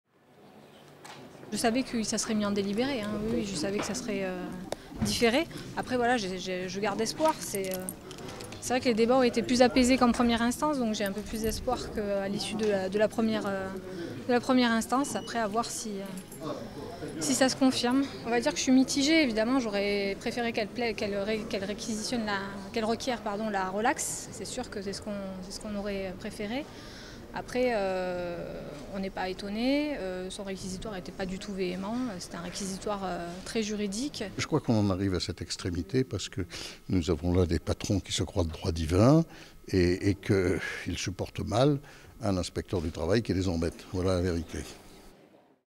Procès en appel de l'affaire Téfal : délibéré le 16 novembre
On débute ce journal avec l’affaire Tefal. On l'a évoqué dans notre édition de mercredi. Le procès en appel de Laura Pfeiffer, ancienne directrice du travail chez Tefal et Christophe Micheli lanceur d'alerte s'est déroulé mercredi après midi à Chambéry. En décembre dernier, Laura Pfeiffer avait été condamnée à 3500€ d'amende avec sursis pour recel et violation du secret professionnel. Christophe Micheli, informaticien, avait lui écopé de la même peine pour accès frauduleux. Dans son réquisitoire, l'avocate générale a demandé la confirmation de ces peines. Le jugement a été mis en délibéré. Il sera rendu le 16 novembre prochain.